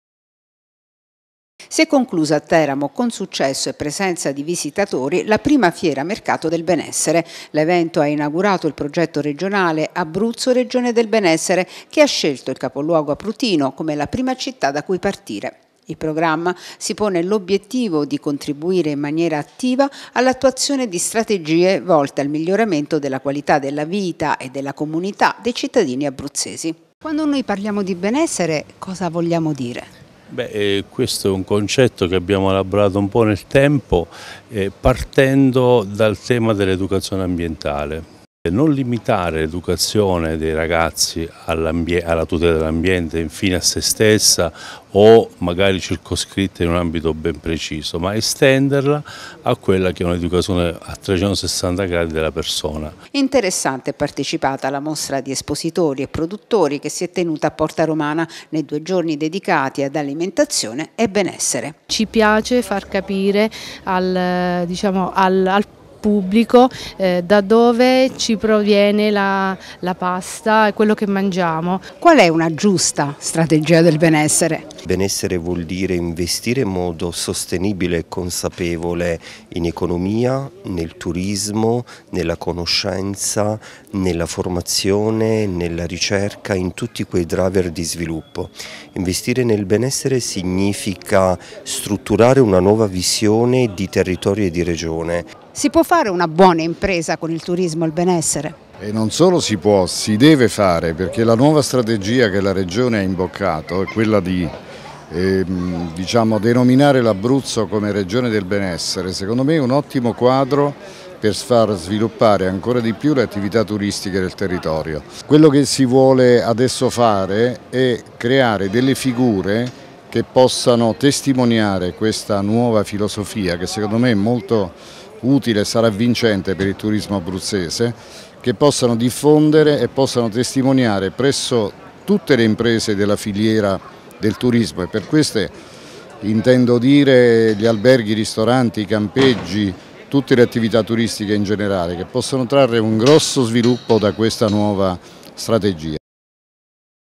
Teramo. Successo per la prima fiera del benessere
Si è appena conclusa la prima fiera del benessere realizzata a Teramo lungo Porta Romana. L'evento si inserisce nel progetto regionale Abruzzo Regione del Benessere che mira a migliorare la qualità della vita degli abruzzesi